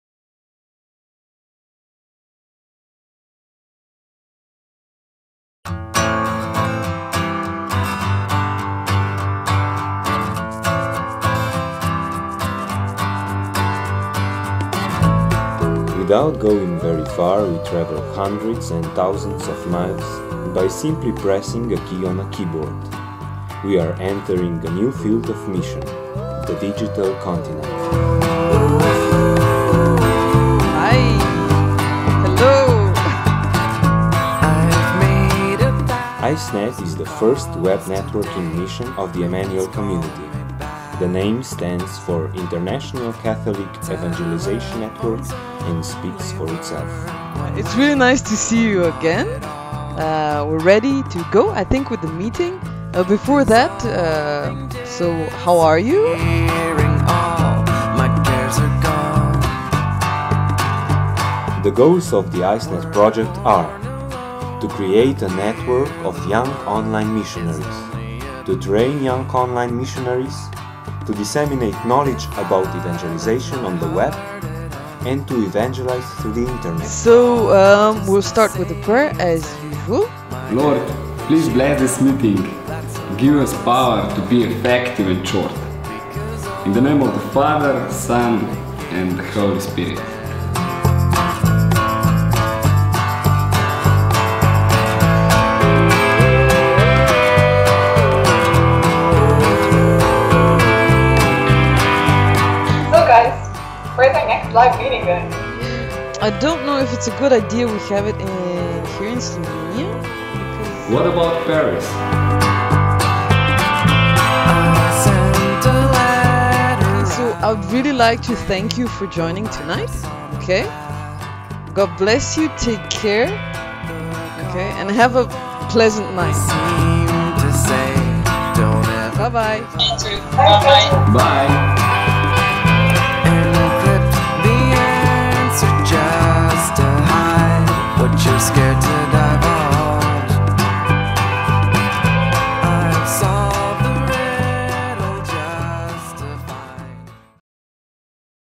ICEnet promo
A video about ICEnet: what it is, what we do...
ICEnet is a web networking mission of the Emmanuel Community. The name of the project stands for International Catholic Evangelization Network. The goals of the ICE-net project are to evangelize on the net, to train young people to be missionaries on the web, to create a collection of media products and to set up a network of missionaries in the world.